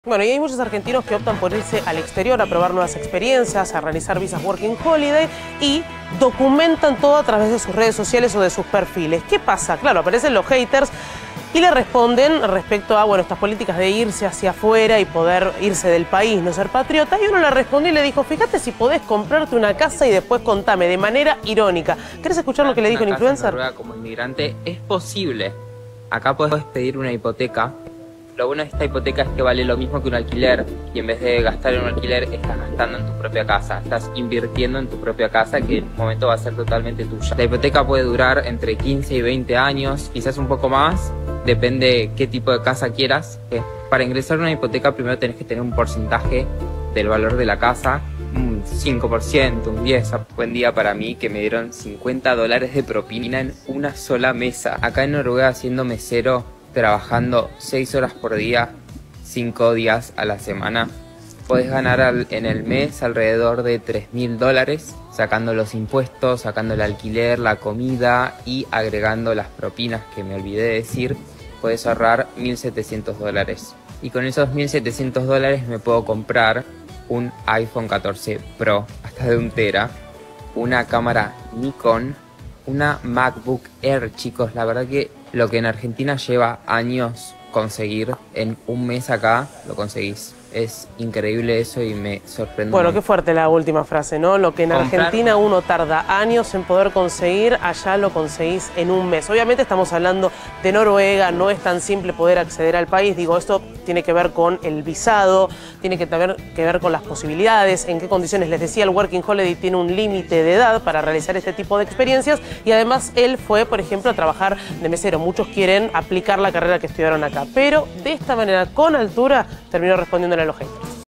¿Cuánto GANA un argentino en NORUEGA?
Aproximadamente trabajando como mozo, 6hs al día, 5 días de la semana se puede ahorrar hasta 1700 dólares mensuales.